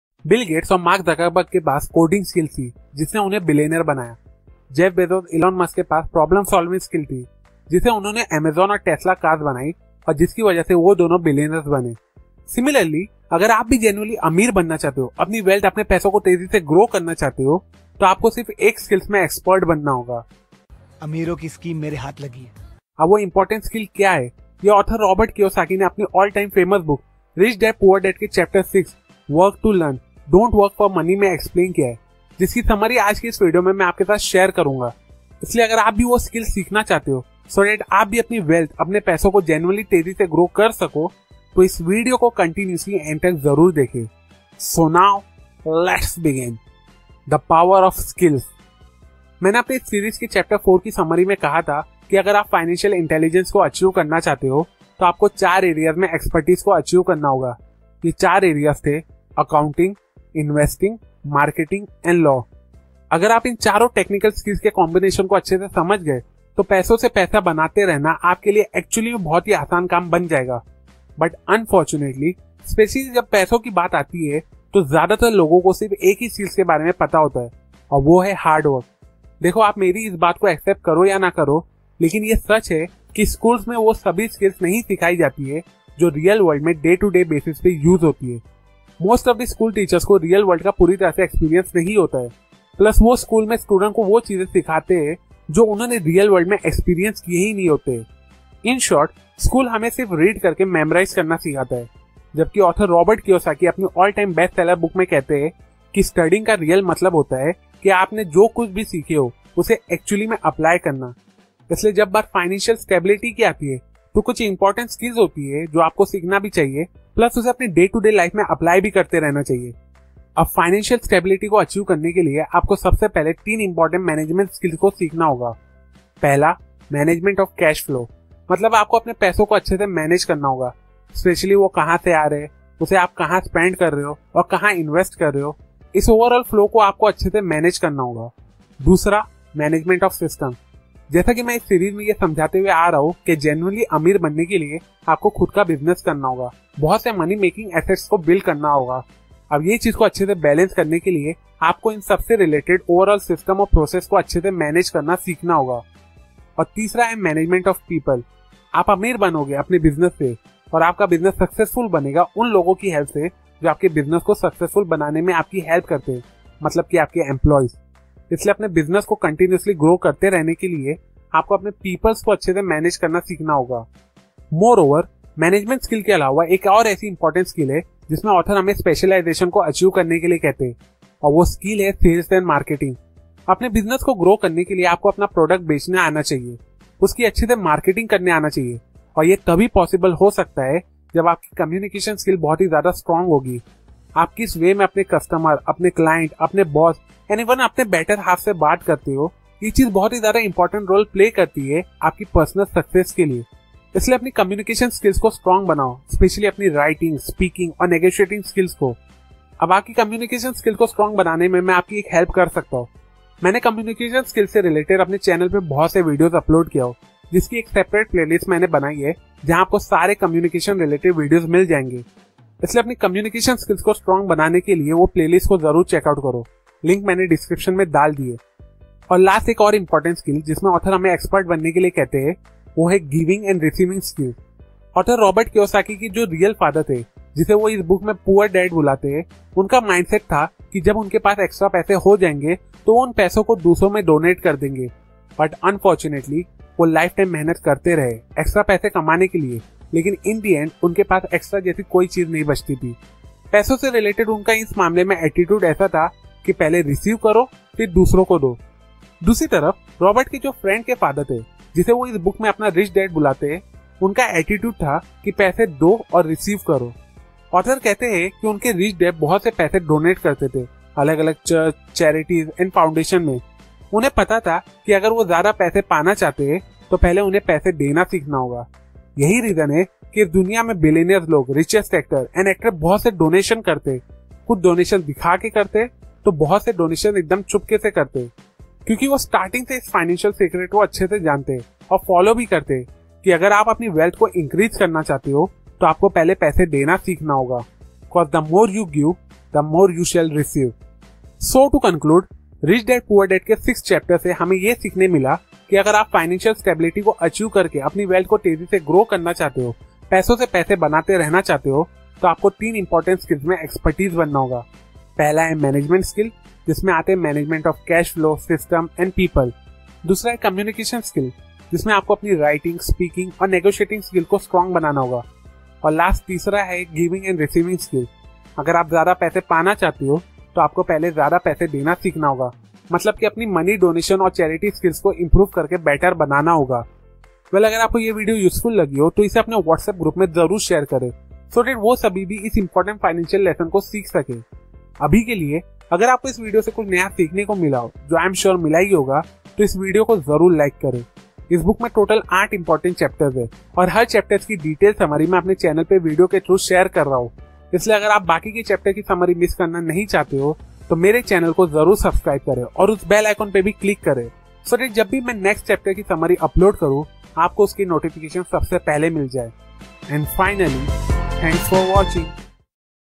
RICH DAD POOR DAD | CHAPTER 6: WORK TO LEARN - DON’T WORK FOR MONEY | SUMMARY (HINDI) | Mr EuS
IMPORTANT LESSONS FROM THIS VIDEO:

1) THE POWER OF SKILLS (0:47)
2) 3 IMPORTANT MANAGEMENT SKILLS (2:00)
3) 2 SKILLS TO GROW YOUR MONEY (2:54)